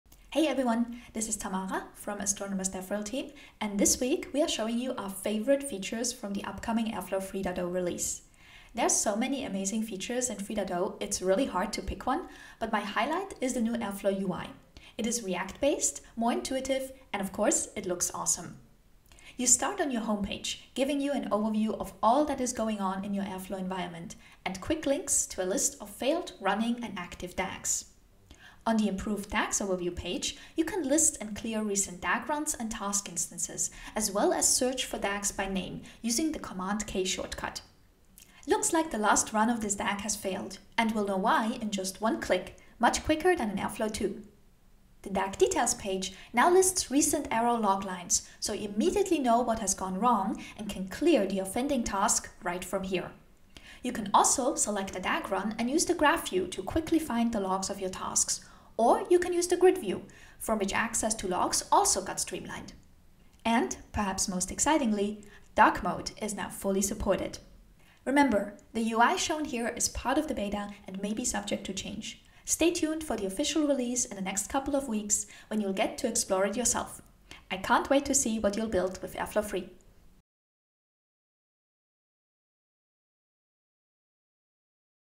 Airflow 3.0 pre-release: new UI
Hear what Astronomer's DevRel team is most excited about in the upcoming Airflow 3.0 release. In this video: the new UI! To learn more about everything in the release, join our webinar